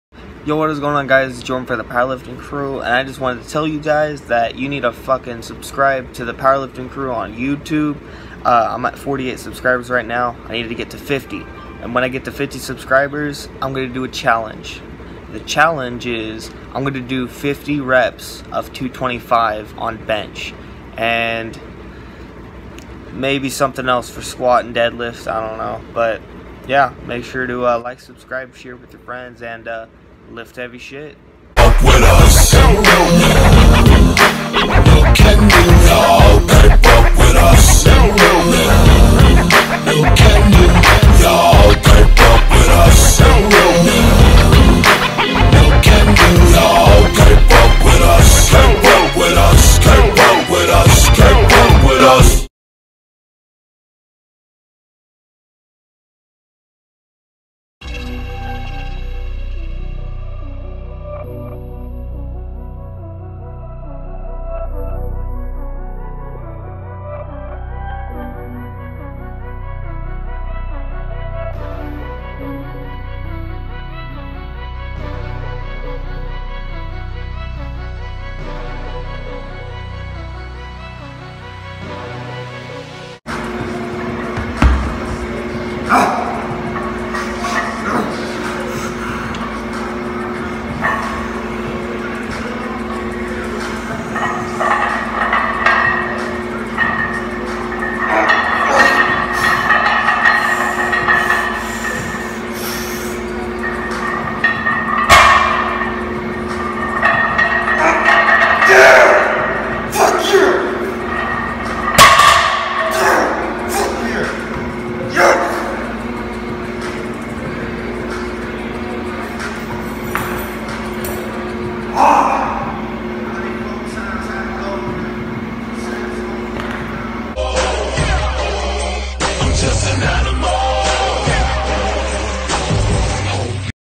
A Little Too Late... | Heavy Deadlift Max Day
Jordan The Maniac Goes To Anytime Fitness By Himself And Deadlifts HEAVY ASS WEIGHT!!! Maxim Out.